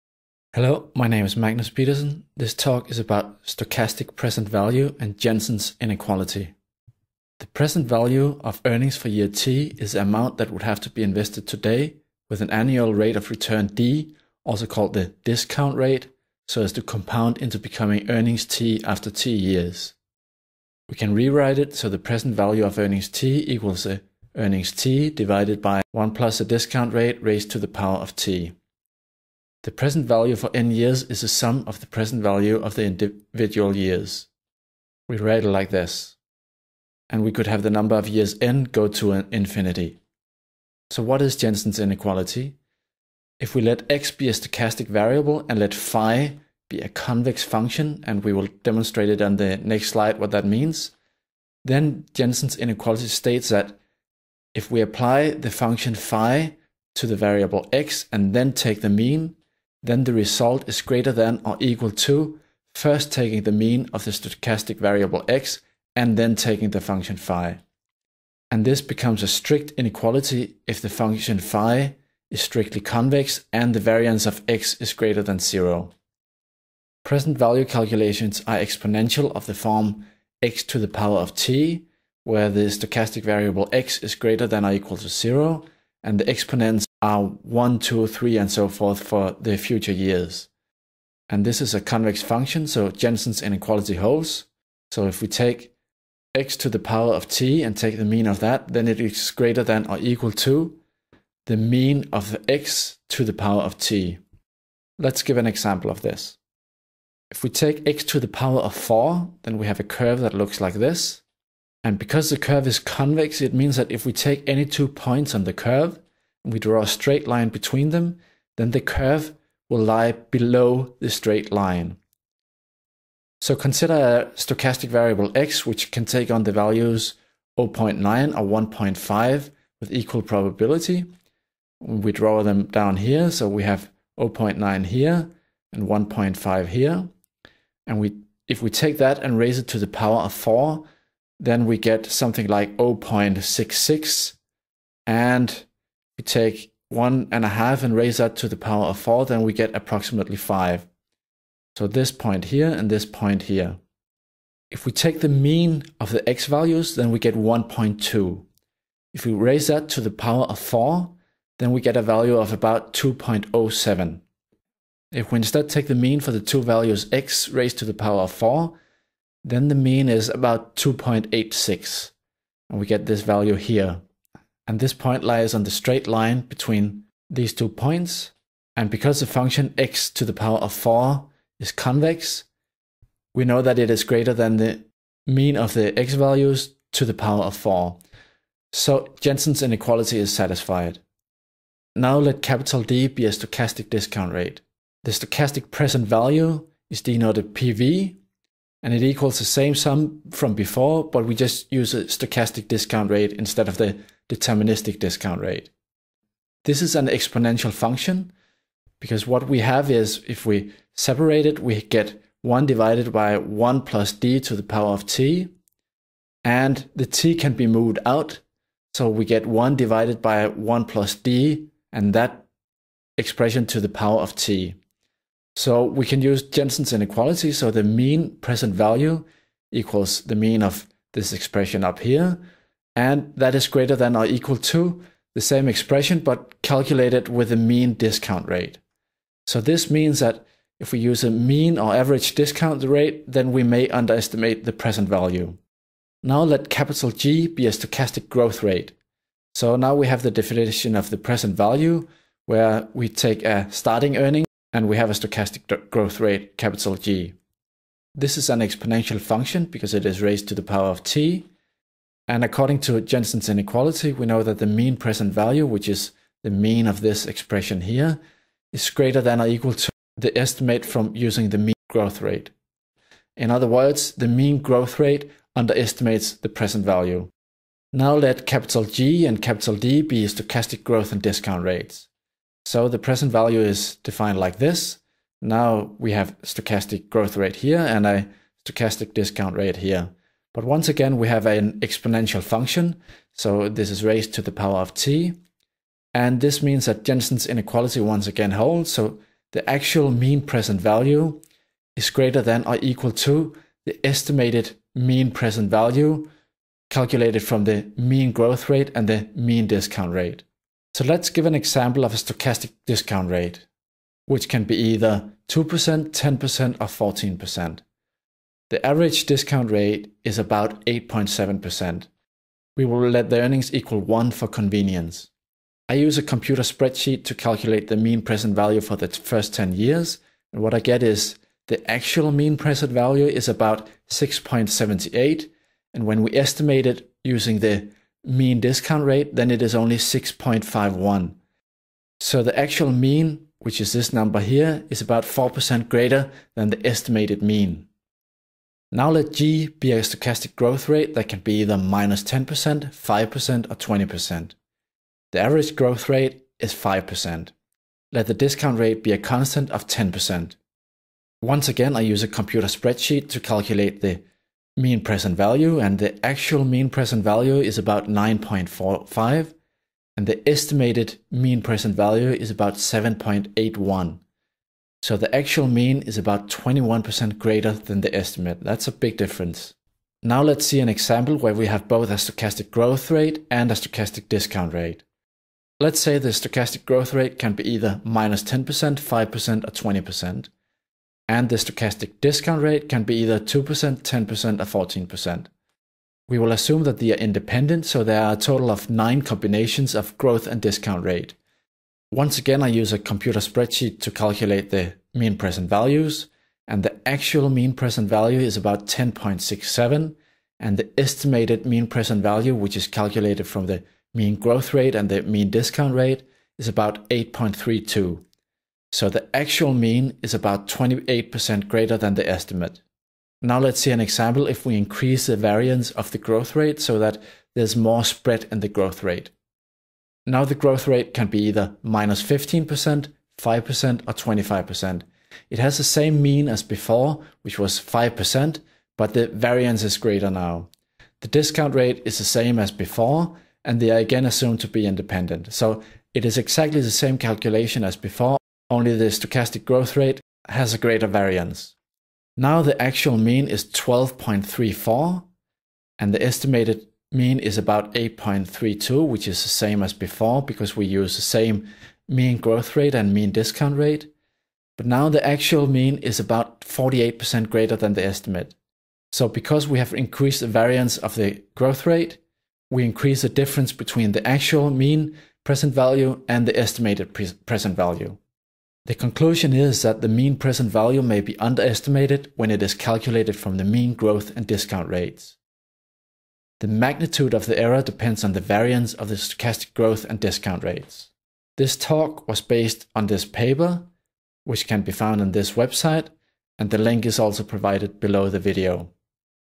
Stochastic Present Value & Jensen's Inequality
The average present value is underestimated when calculated from the average growth and discount rates. This follows from Jensen's Inequality.